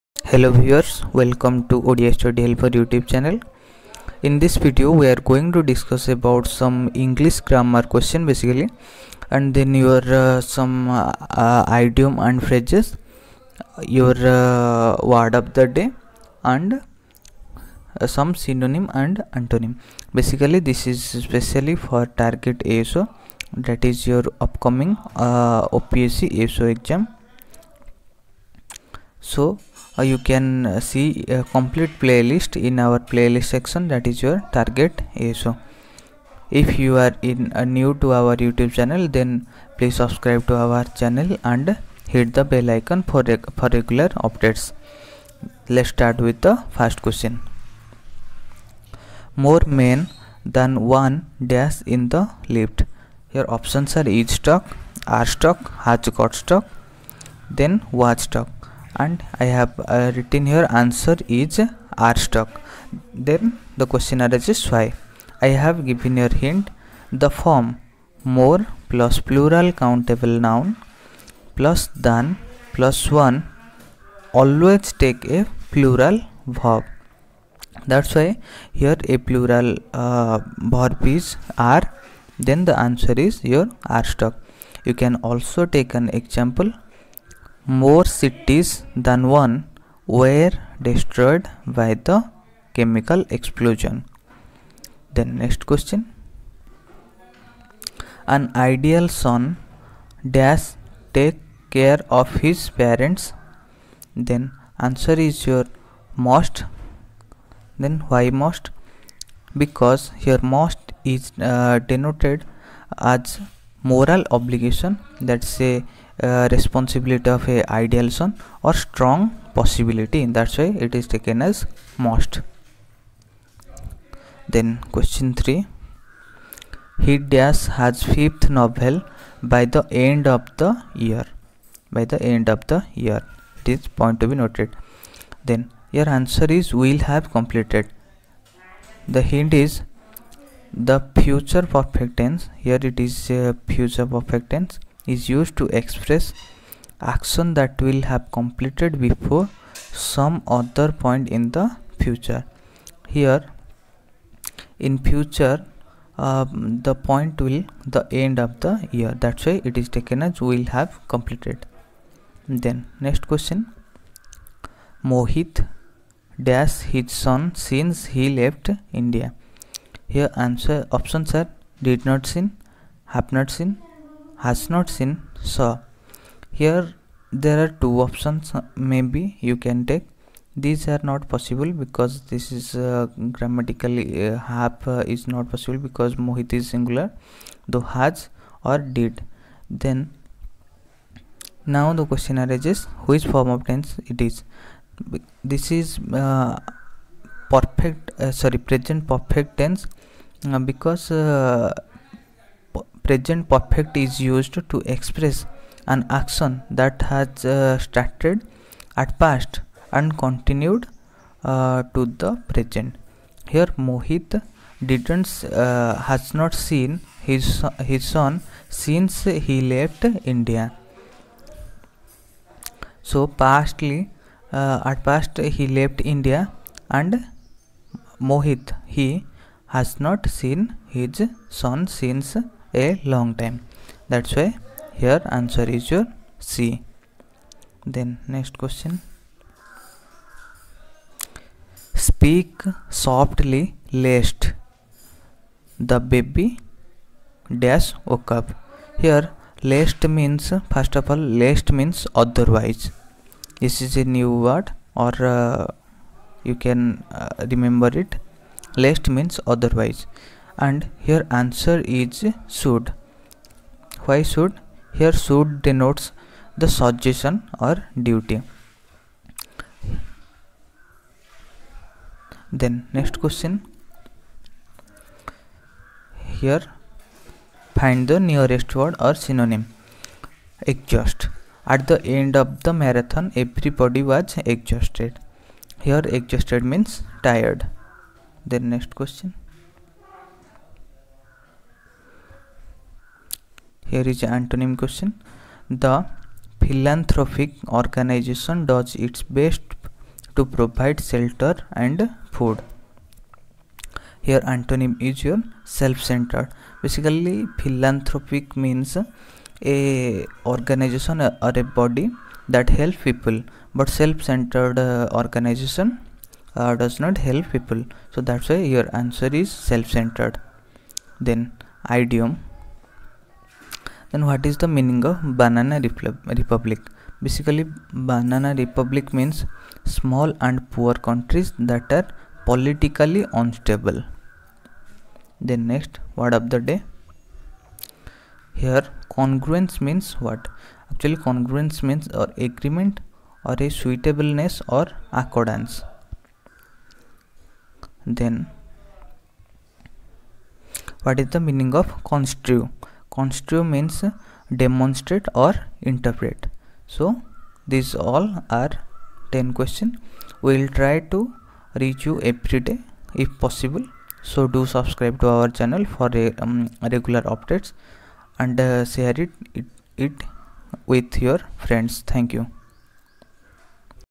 OPSC ASO RECRUITMENT 2021 || ASO English Grammar Question
This video is help full for OPSC ASO  English. This video contains English grammar, Synonym, antonym, Idioms, Phrases, Words of the day.

Download Textbook app for ASO test series